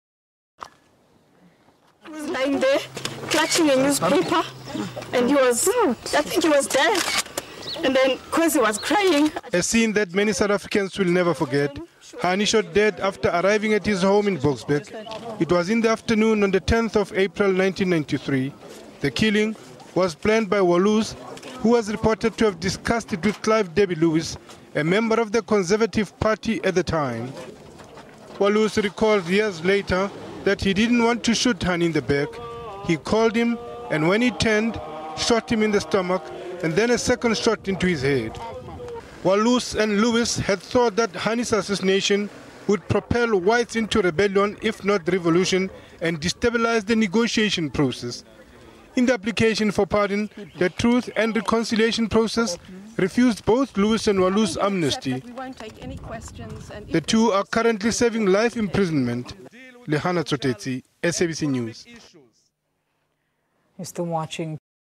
10 April 2014 marks 21 years since Hani's murder
Thursday April 10, marks 21 years since charismatic anti-apartheid leader, Chris Hani, was gunned down by Polish Immigrant Janusz Walus. Hani died instantly. 21 years later questions are still being asked as to why Walus killed Hani who had become an important player in the country's negotiations processs.